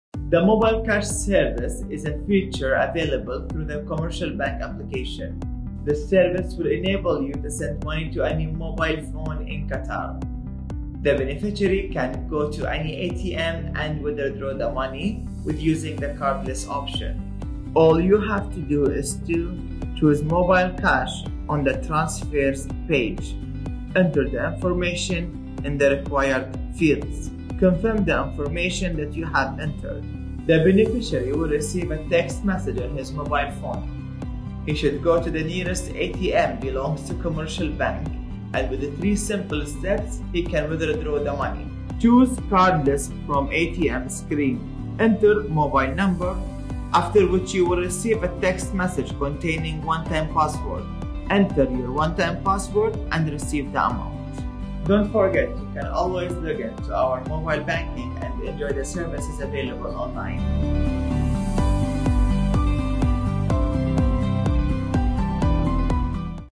Mobile cash service - Transfer and receive money instantly from your mobile phone
Transfer and receive money instantly from your mobile to your kids, family, friends or even yourself.